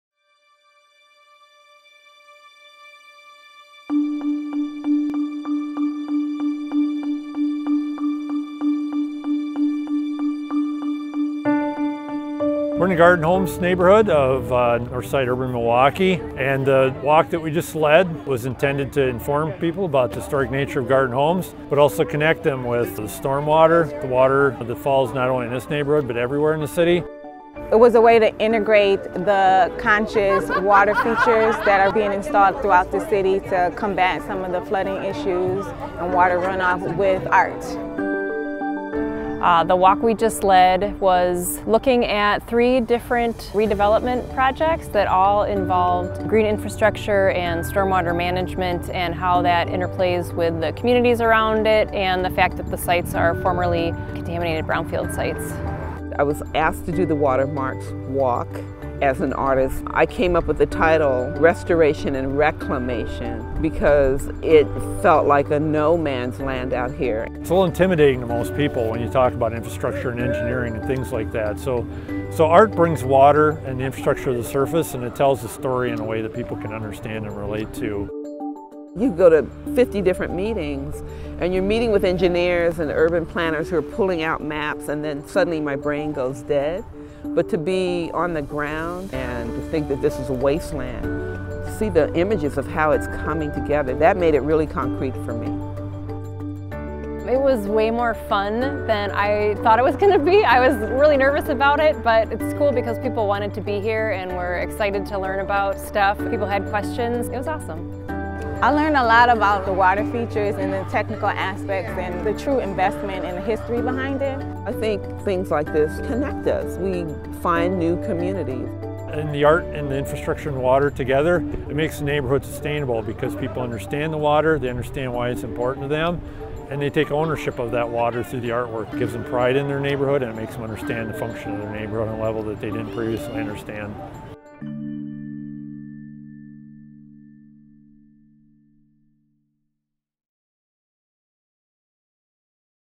WALKS WaterMarks May 2019
Connecting Water and Art in the Garden Homes
Restoration and Reclamation in the 30th Street Corridor

WaterMarks is an initiative to develop an inclusive and urban-scaled vision for the city of Milwaukee, designed to help its citizens better understand their relationships to the water systems and infrastructure that support their lives. This is envisioned as a multi-layered framework that can be implemented over time.
As part of the WaterMarks initiative, WaterMarks has created a distinct YouTube channel to showcase its VOICES.